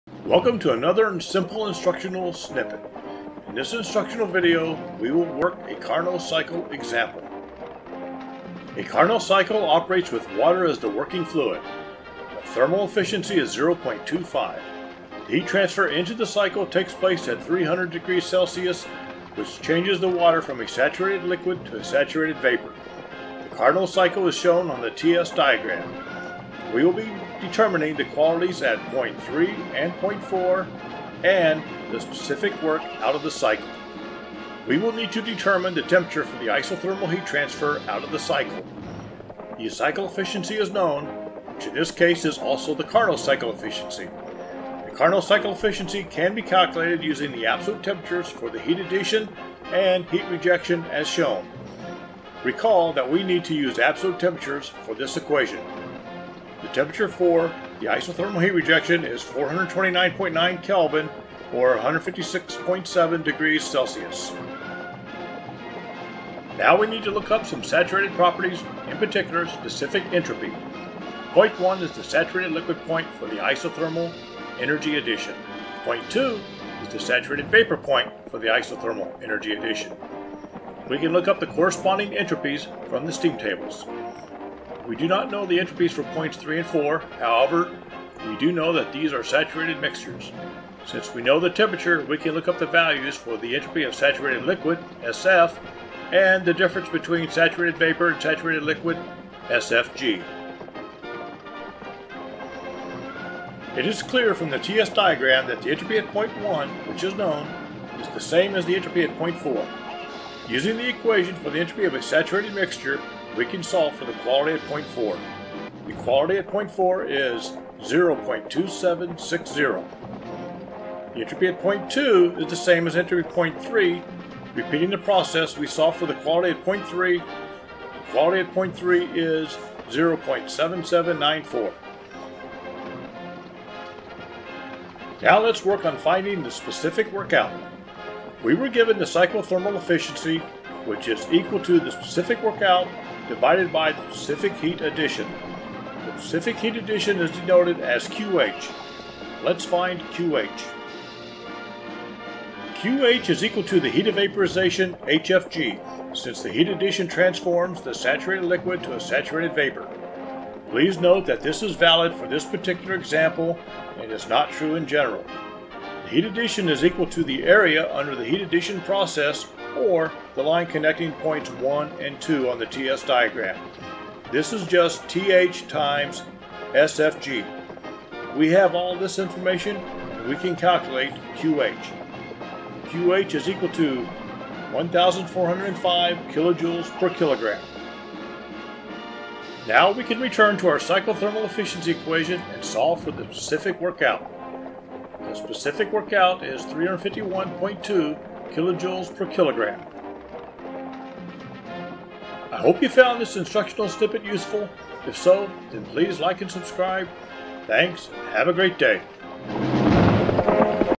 CarnotCycle Example
Carnot cycle example with saturated water as the working fluid. Thermodynamics, thermo, 1stLaw, Mechanical Engineering.